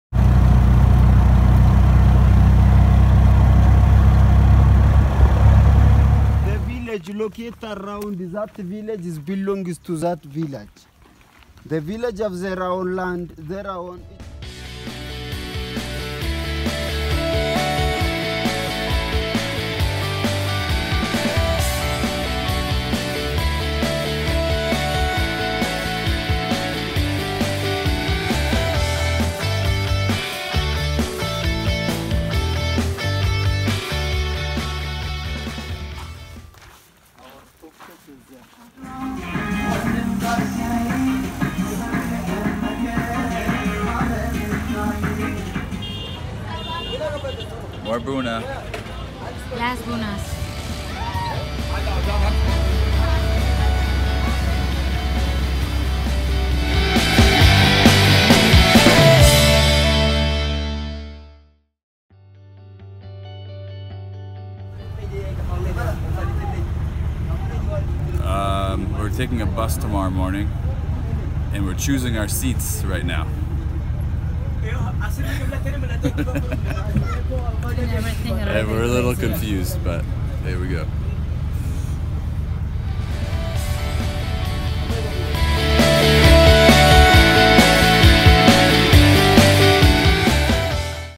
VLOG10 : Ethiopia to South Africa - Bicycle Touring Africa
VLOG 10 : Ethiopia to South Africa -  Matt and Larisa rode their bikes from Barcelona, Spain to Dakar, Senegal between September 20th, 2017 and January 20th, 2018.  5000 km (3000 miles).  They then flew to Addis Ababa, Ethiopia with their bikes and wanted to share more of their journey.  Each day they are making a short video to share their day on the road.  This is day ten of this six month leg of the journey.  Video filmed and edited on February 4th, 2018.